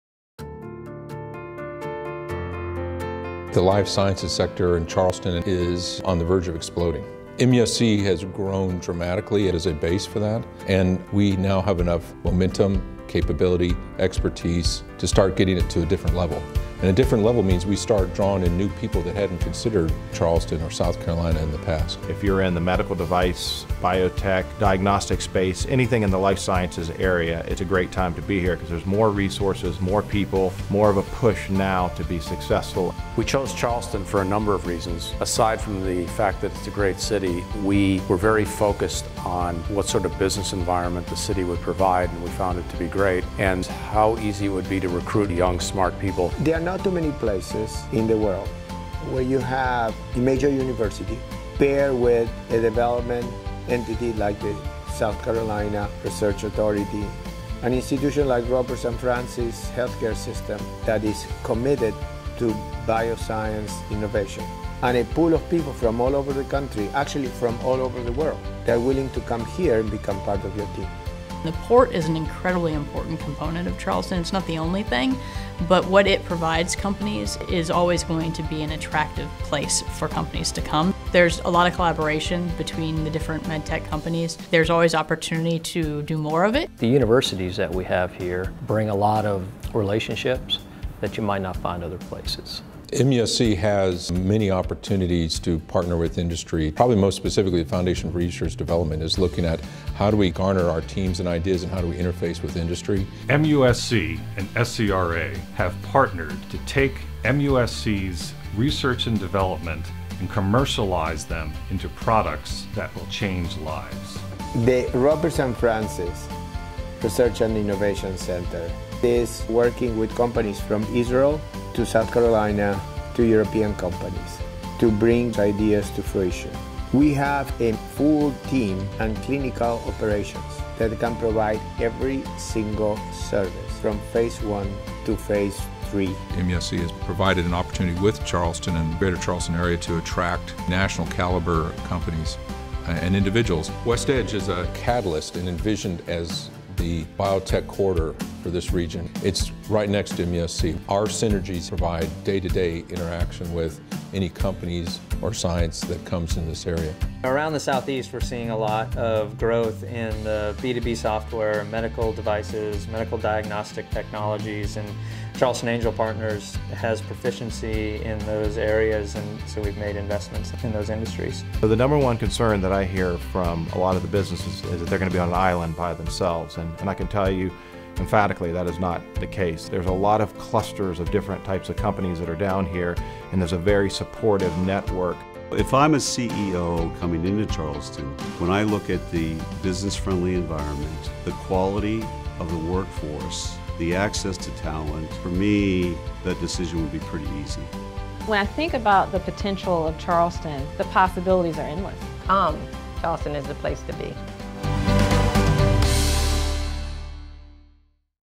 Life Sciences Charleston | SC | USA
The Charleston region offers medtech companies an adaptable workforce with a business-friendly environment. The presence of the Medical University of South Carolina, a tier 1 academic and research institution, the Roper St. Francis Research and Innovation Center, our proximity to the Port of Charleston, and our ability to attract talent make this an ideal location for medtech companies looking for growth.